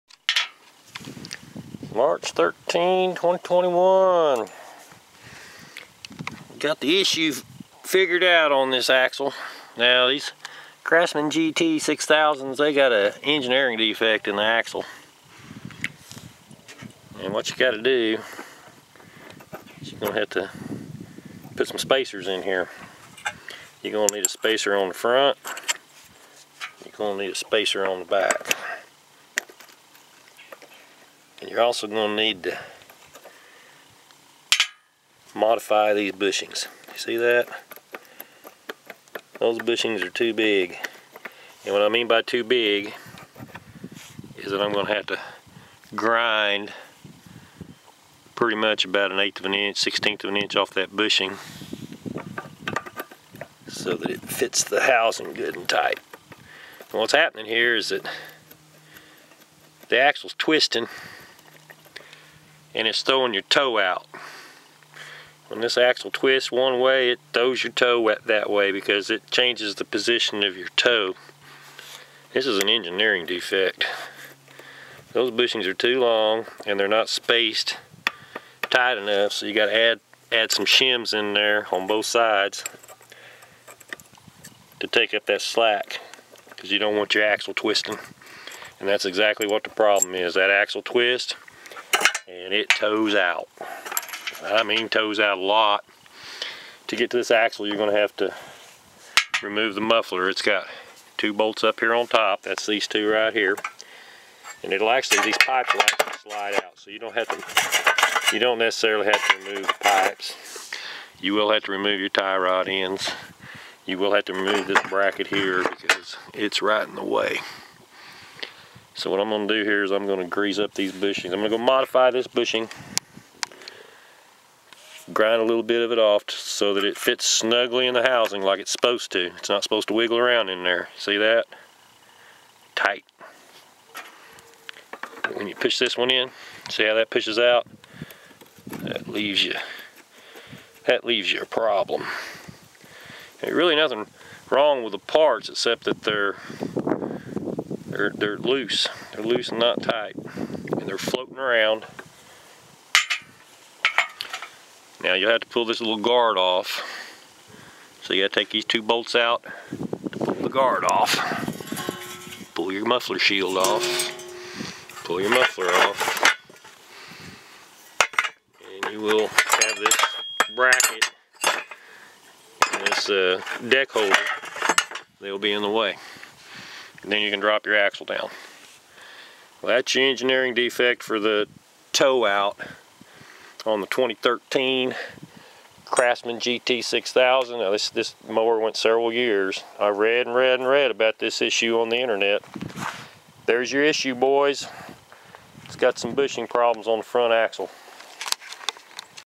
Craftsman GT 6000 Toed Out Problem Resolved. 2013 Model. Engineering Defect. Pt.1
Bought me a Used Craftsman GT 6000 and I've been working on it.  It has/had quite a few Problems.  This Vid concerns the Toed Out Issue.  A lot of Vids out there of People Guessing about why their Mower is Toed Out. If You grab the both Front Tires and push and pull them in and out the Axle will walk front to back/twist.  That's because there is Excessive Play in the Axle Bushings Area that causes the Toe on the Front Tires to severely change.  There might be some other Issues with the Front End but the Axle Bushings are Defectively Engineered.